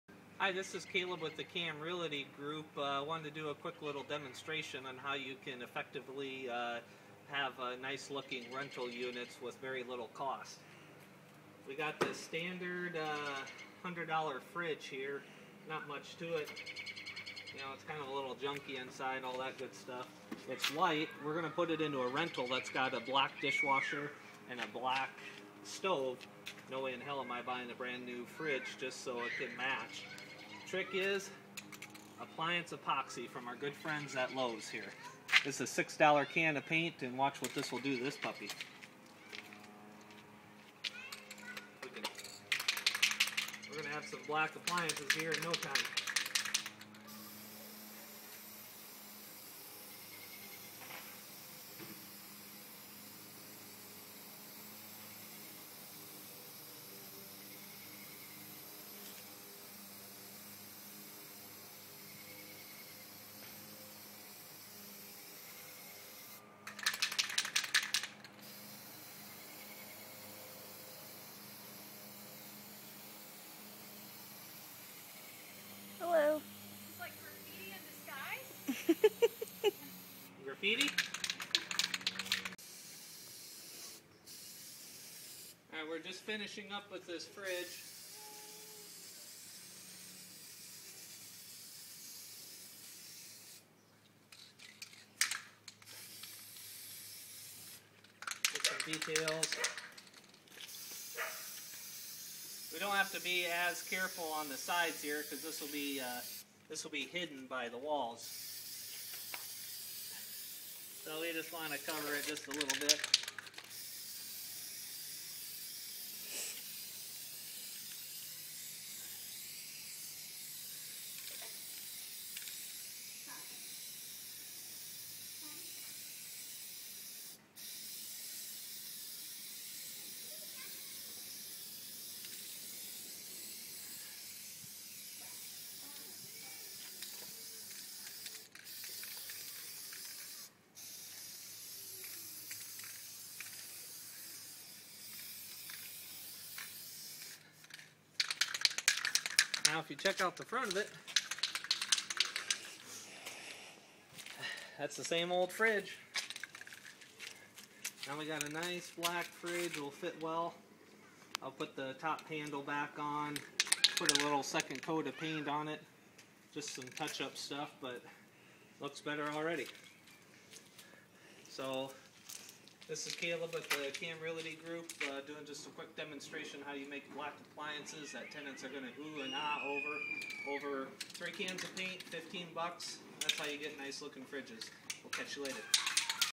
Plain fridge to Black fridge
Most people prefer black appliances over white appliances and or cream colored appliances. If you want black appliances, that's easy, there's specifically designed black appliance paint that works great, check it out!